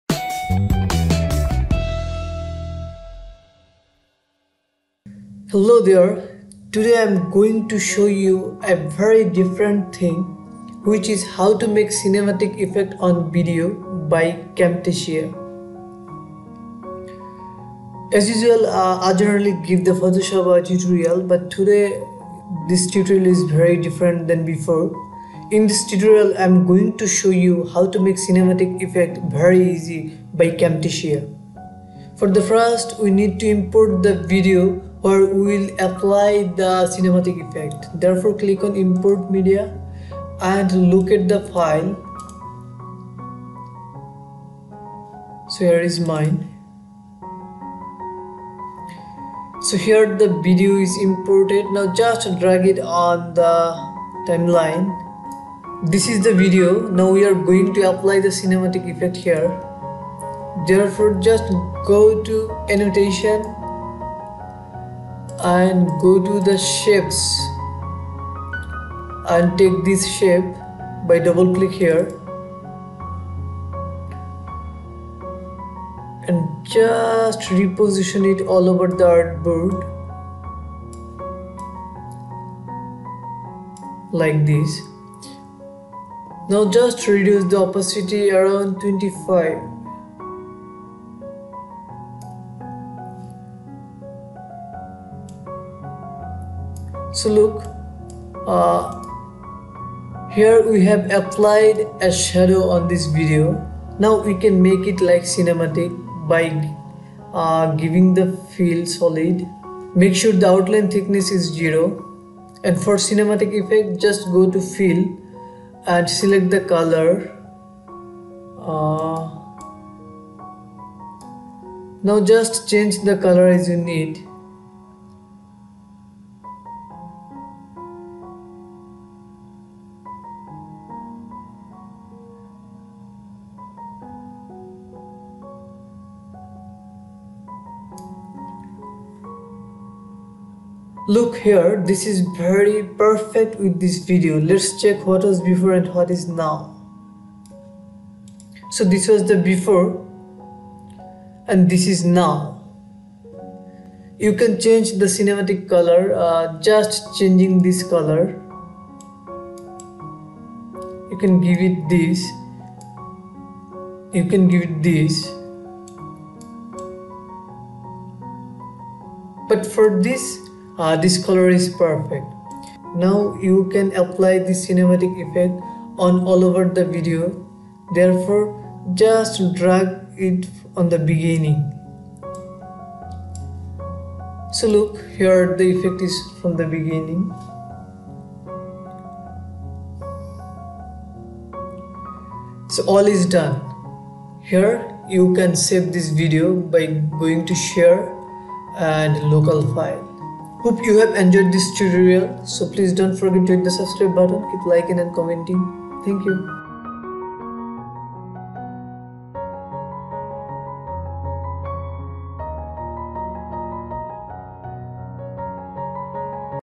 How to Make Cinematic Color Effect On Videos Easily By Camtasia : Camtasia Tutorial
Hello Viewers,
Today I have come up with a very new thing.
It may helps to them Who want to Make Cinematic Video effect on Videos.
Here I will show How to Make Cinematic Color Effect On Videos Easily By Camtasia : Camtasia Tutorial.

Skype: rakibhafiz
Follow me on